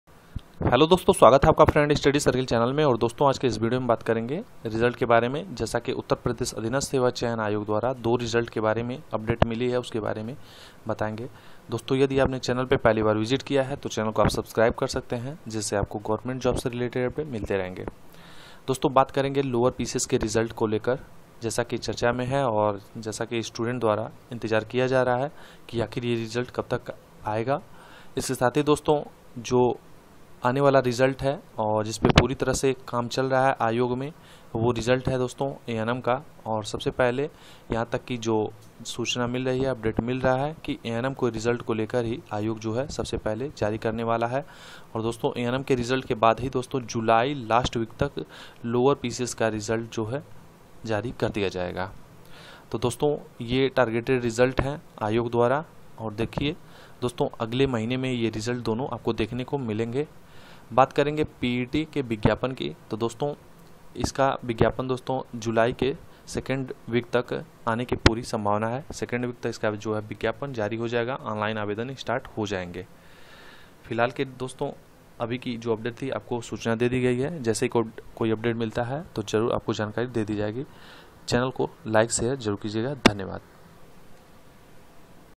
UPSSSC LOWER PCS RESULT कब तक आएगा II ANM RESULT PET 2022 NOTIFICATION
HELLO FRIENDS WELCOME TO FRIENDS STUDY CIRCLE CHANNEL

DOSTO IS VIDEO ME UPSSSC KE LOWER PCS KE RESULT KE EXPECTED DATE KE BARE ME JANKARI DI GAYI HAI.